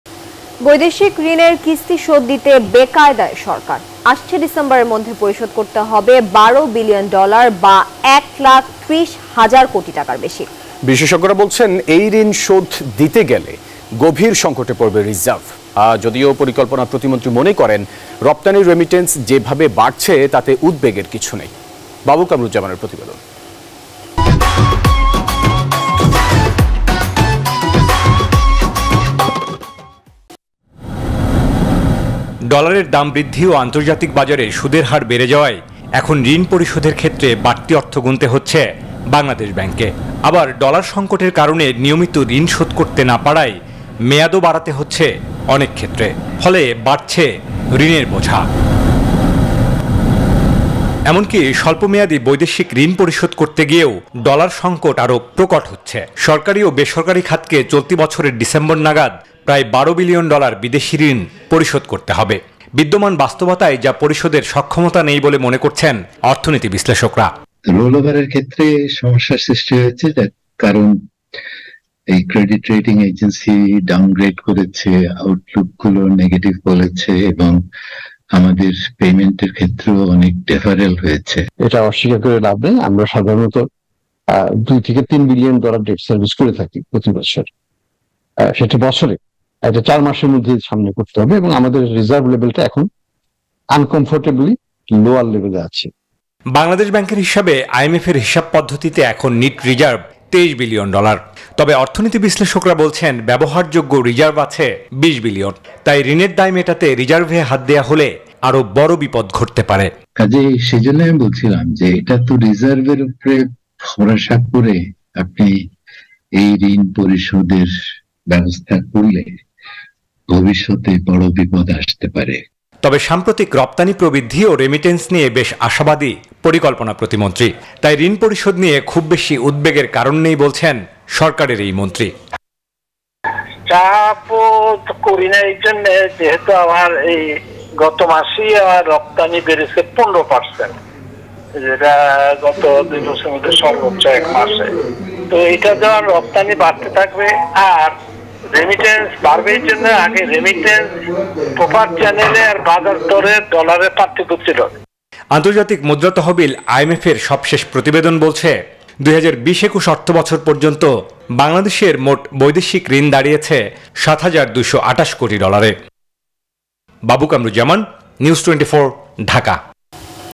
বৈদেশিক ঋণের কিস্তি শোধ দিতে বেকায়দায় সরকার! | Government | Debt | Economy | News24
NEWS24 Team